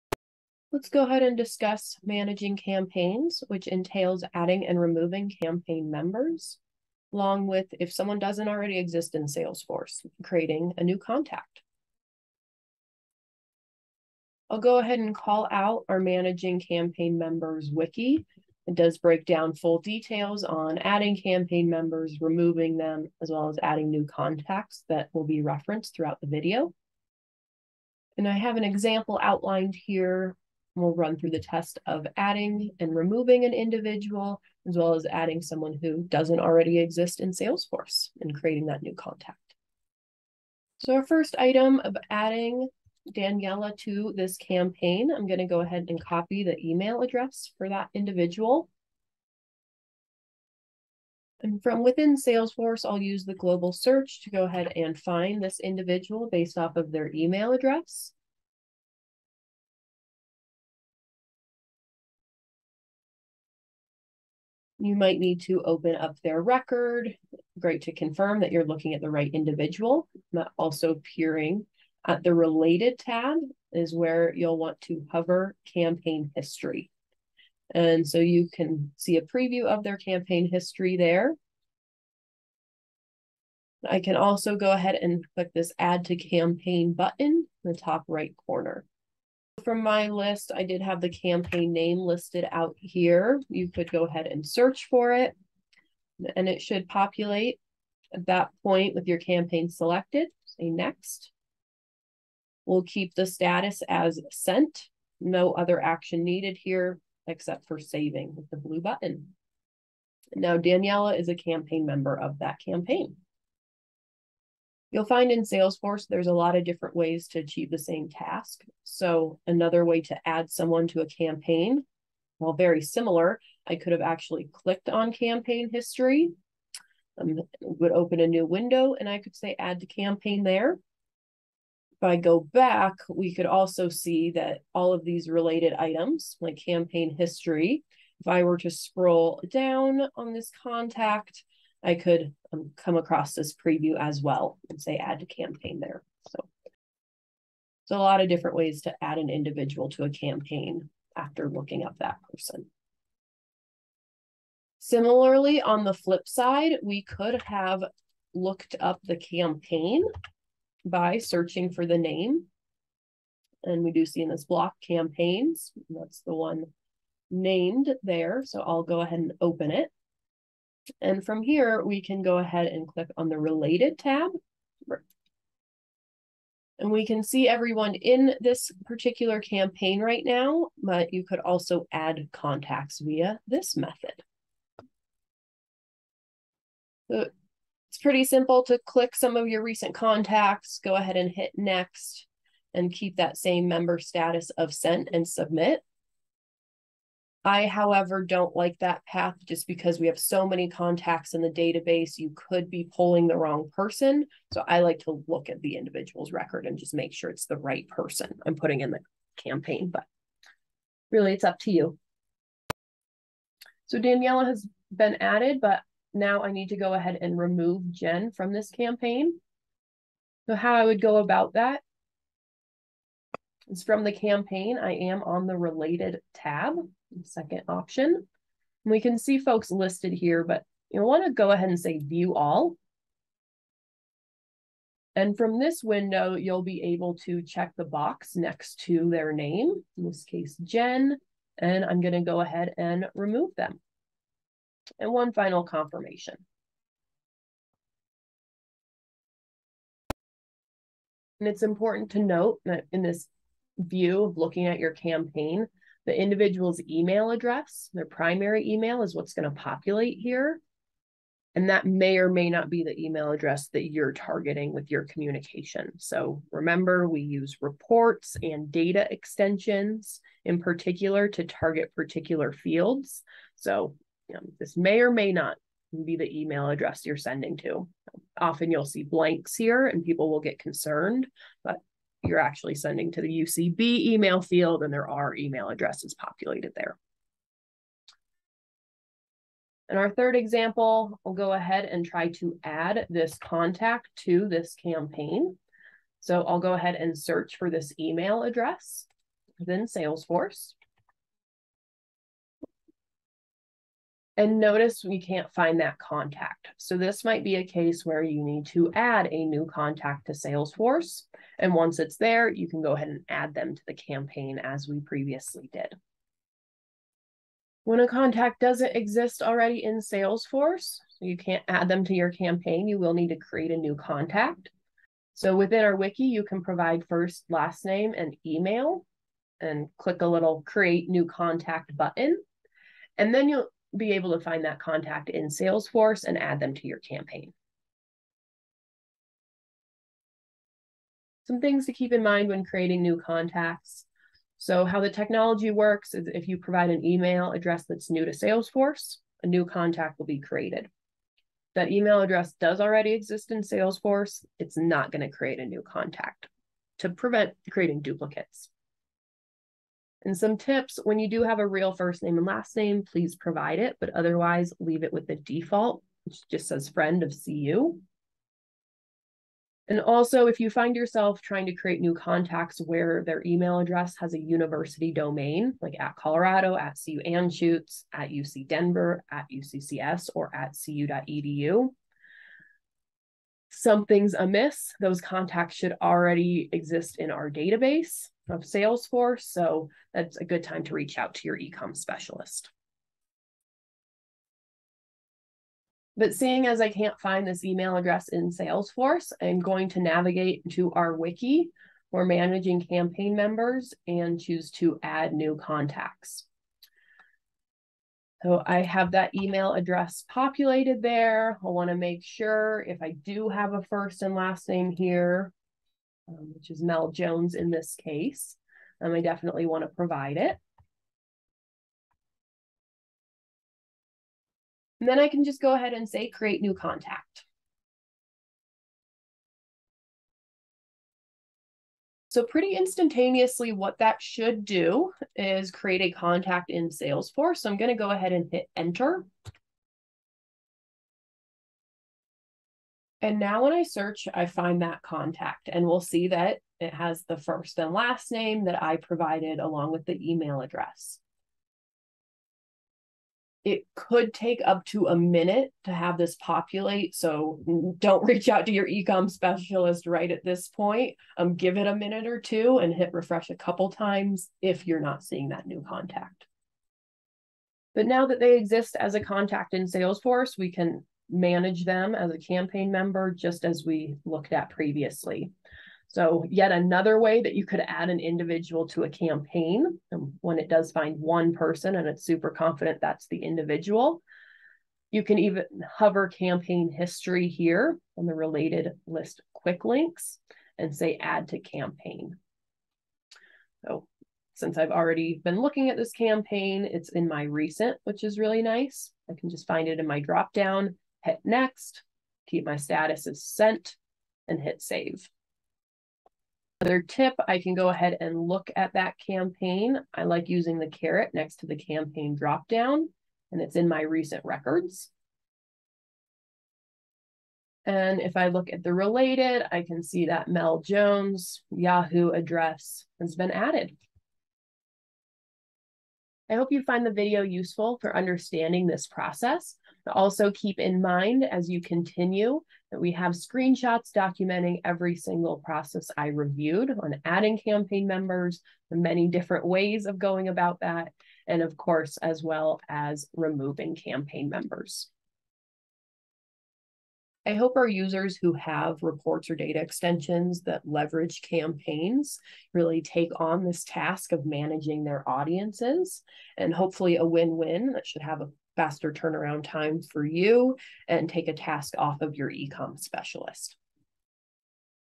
Salesforce | Managing Campaigns
0:00 | Overview
0:40 | Add Campaign Members
3:35 | Remove Campaign Members
4:50 | Add New Contacts
8:47 | Closing